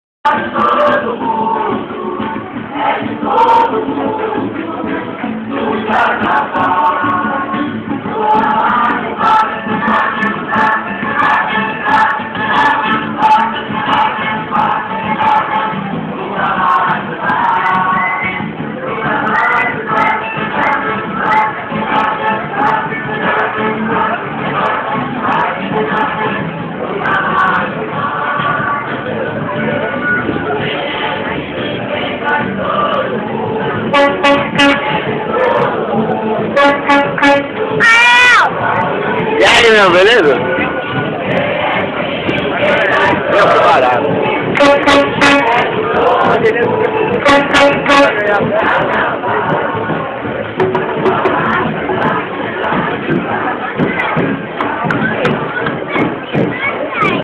São Luís do Paraitinga 2008 - Bonecões
Tradicional desfile de blocos com bonecos gigantes, em S. Luís do Paraitinga. Carnaval 2008, do Balacobaco!